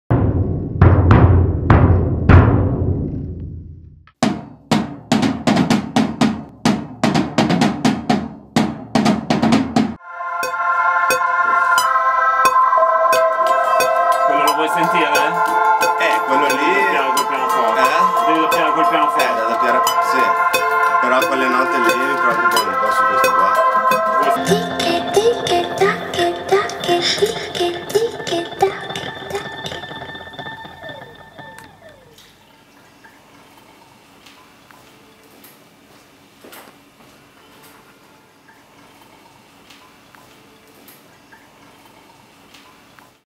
VideoBlog #5 - Drums and Guitars
Working on new album with Roberto Vernetti, Cristian Milani and Michele Clivati.